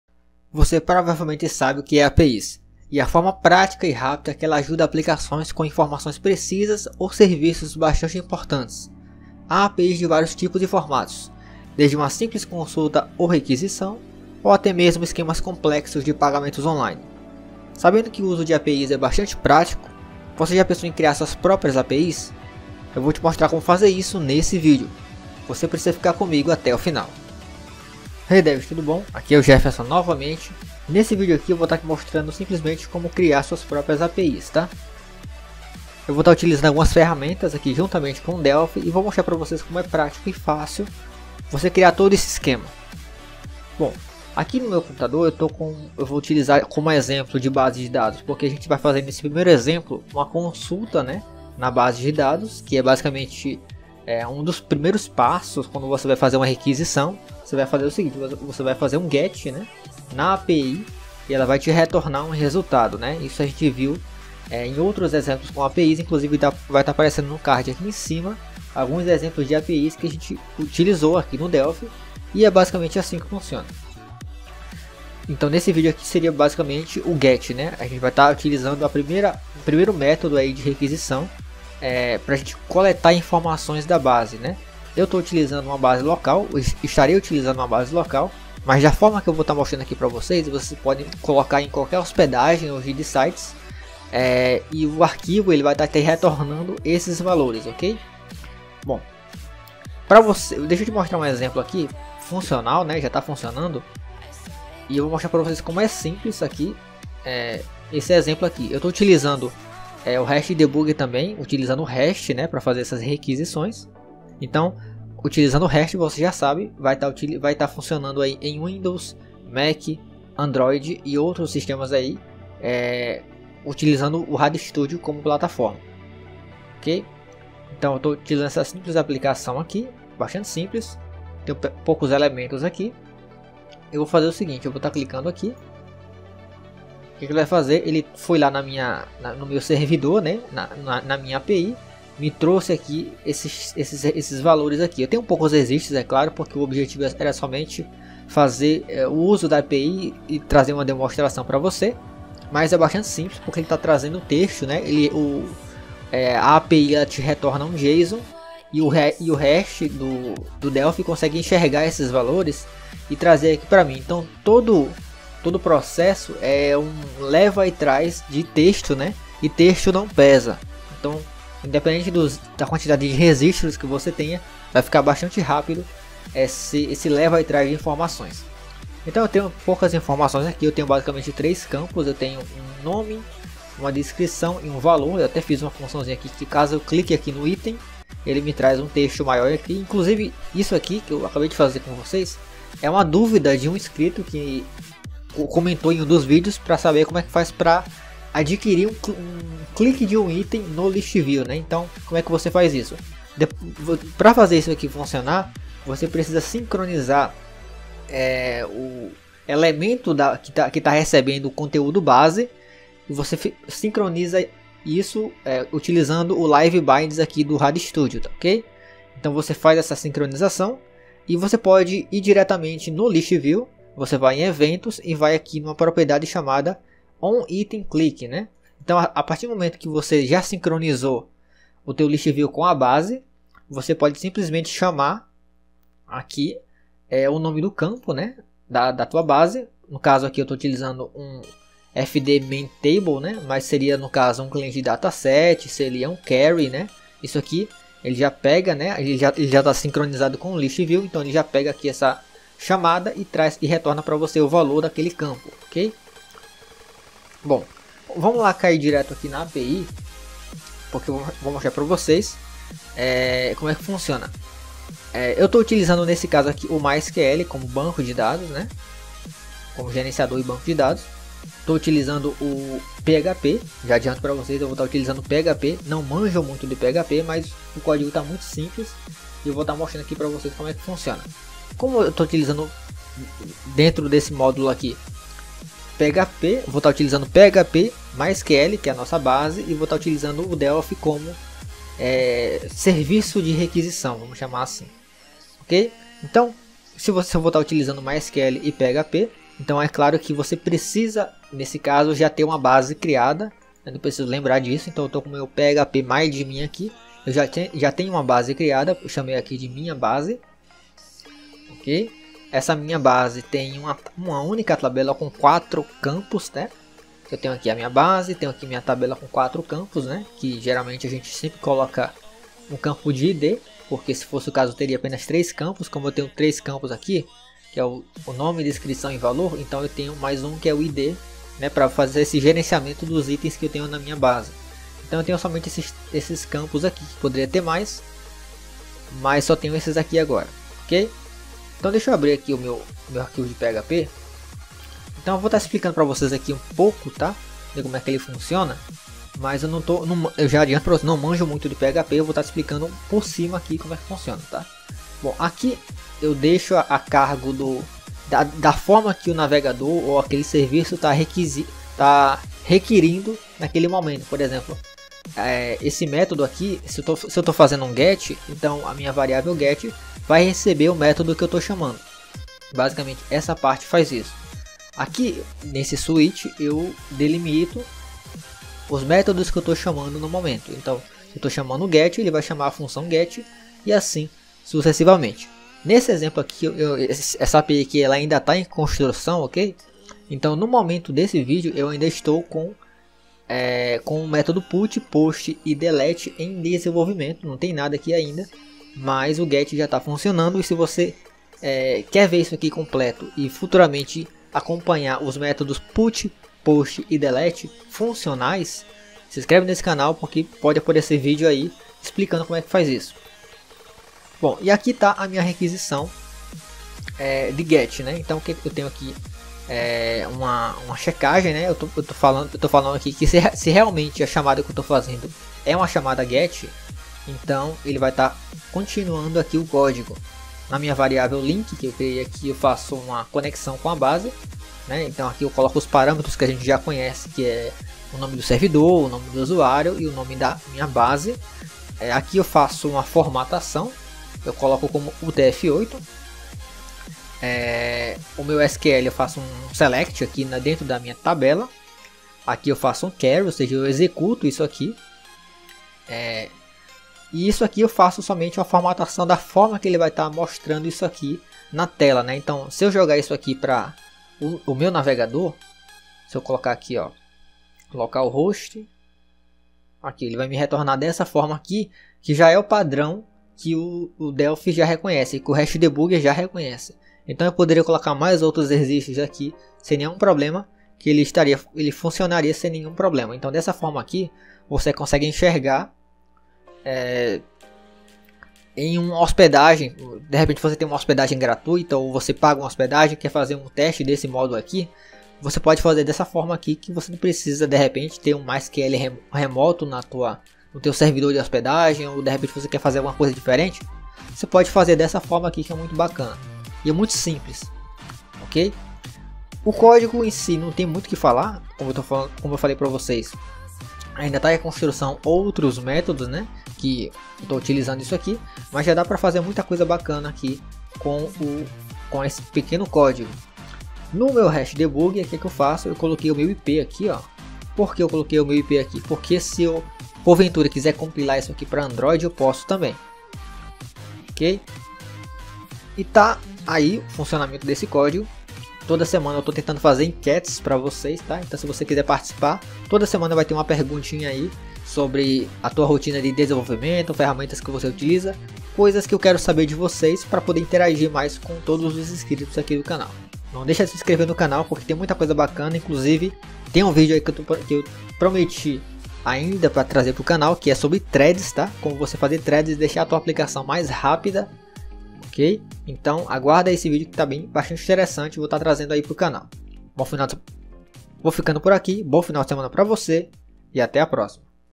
[PASSO-A-PASSO] Crie Sua Própria API e Faça Requisições Com o Delphi! [Com 1 Arquivo Apenas]
Você já pensou em como criar suas próprias APIs e fazer requisições com o delphi? Você sabe o que é uma  APIs?, e a forma prática e rápida que ela ajuda aplicações com informações precisas ou serviços bastante importantes.

Aprenda agora a como criar sua própria api e faça requisições com um delphi sem ter a necessidade de criar arquivos complexos para isso. Tudo que você vai precisar é de um único arquivo que vai fazer todo trabalho pra você. Você só vai precisar configura-lo uma única vez.

Há APIs de vários tipos e formatos, desde de uma simples consulta ou requisição, ou até mesmo esquemas complexos de pagamentos online. Sabendo que o uso de APIs é bastante prático,  Eu vou te mostrar como fazer isso neste vídeo, você precisa ficar comigo até o final.

⨠ Se Capacite ainda mais! ↵
● Quer criar telas profissionais?

● Já pensou em se tornar um FULLSTACK?